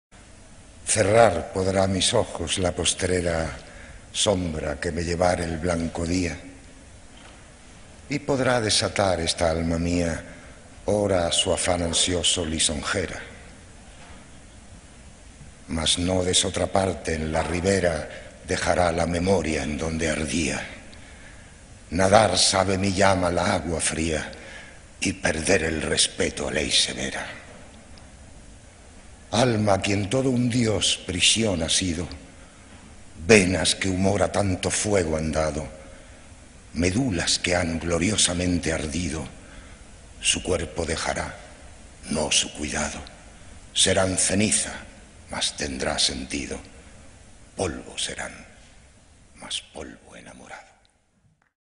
Amor constante más allá de la muerte - Francisco de Quevedo por José Luis Gómez
Amor constante más allá de la muerte

Cerrar podrá mis ojos la postrera
Sombra que me llevare el blanco día,
Y podrá desatar esta alma mía
Hora, a su afán ansioso lisonjera;

Mas no de esotra parte en la ribera
Dejará la memoria, en donde ardía:
Nadar sabe mi llama el agua fría,
Y perder el respeto a ley severa.

Alma, a quien todo un Dios prisión ha sido,
Venas, que humor a tanto fuego han dado,
Médulas, que han gloriosamente ardido,

Su cuerpo dejará, no su cuidado;
Serán ceniza, mas tendrá sentido;
Polvo serán, mas polvo enamorado.

Francisco de Quevedo